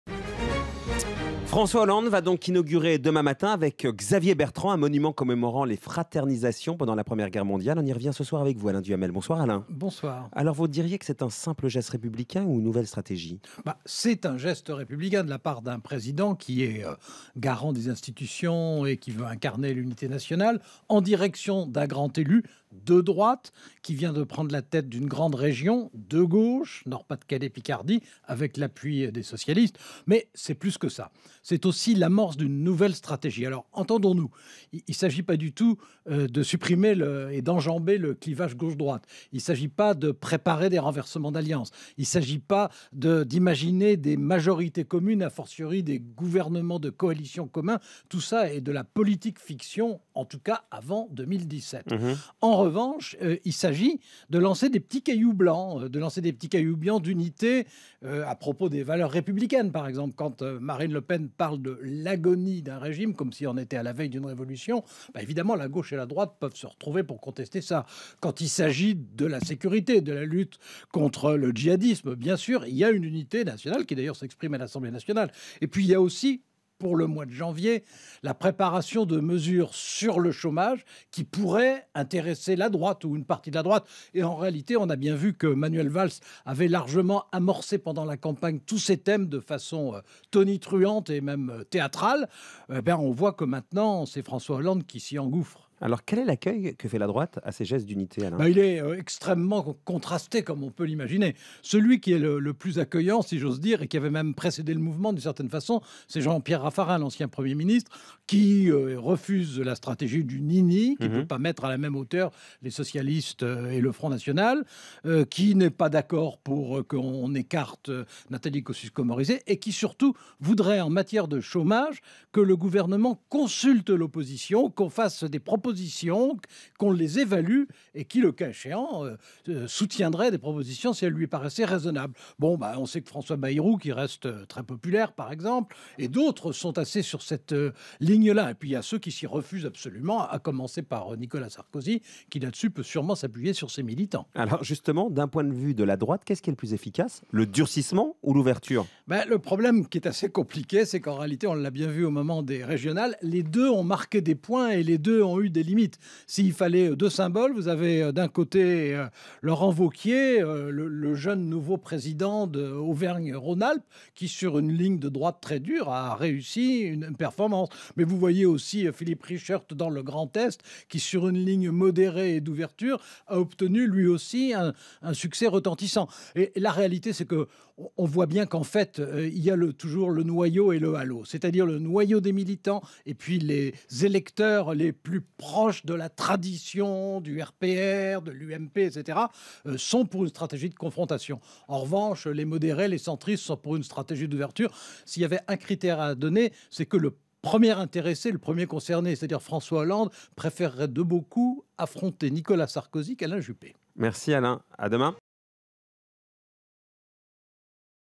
Alain Duhamel : "La stratégie de l'unité nationale" - RTL - RTL
François Hollande va inaugurer jeudi 17 décembre avec Xavier Bertrand un monument dans le Pas-de-Calais.